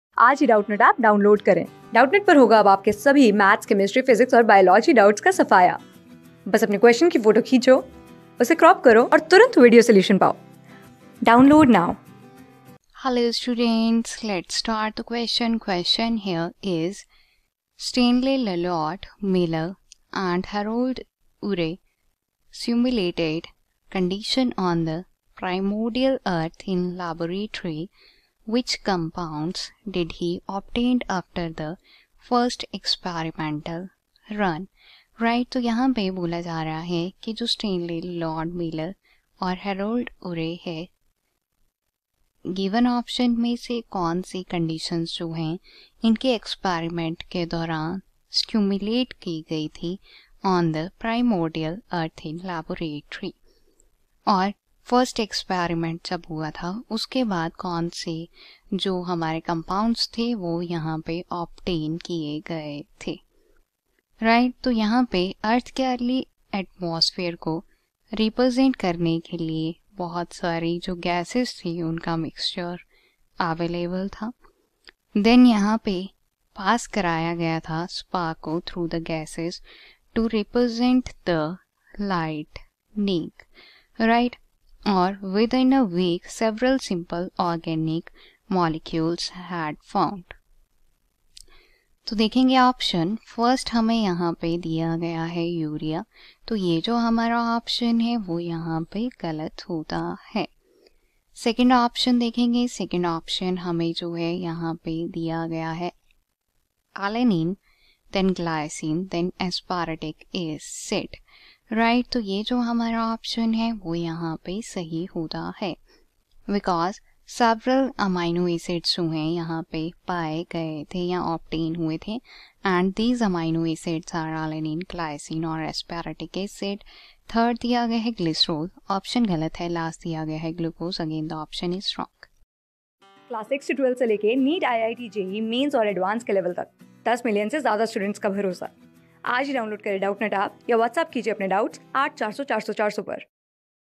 Stanley Lloyd Miller and Harold Urey simulated conditions on primordial earth in laboratory. whi...
Stanley Lloyd Miller and Harold Urey simulated conditions on primordial earth in laboratory. which compounds did he obtained after the first experimental run?
Class: 12
Subject: BIOLOGY
Chapter: TEST 6
Board:IIT JEE

👉 Have Any Query? Ask Us.